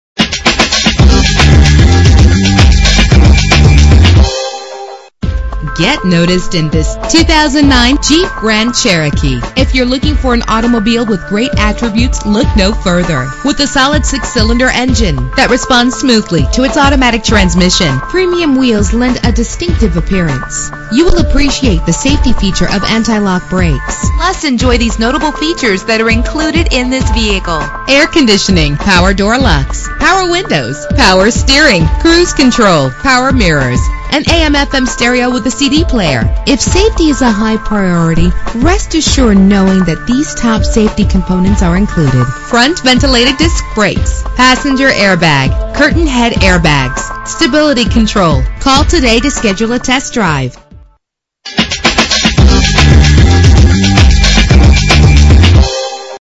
2009 Jeep Grand Cherokee - White Plains NY
Year: 2009
Make: Jeep
Model: Grand Cherokee
Trim: Laredo
Engine: 3.7 liter 6 cylinder 12 valve
Transmission: Automatic
Color: Black
Mileage: 45103
Address:
25 West Post Road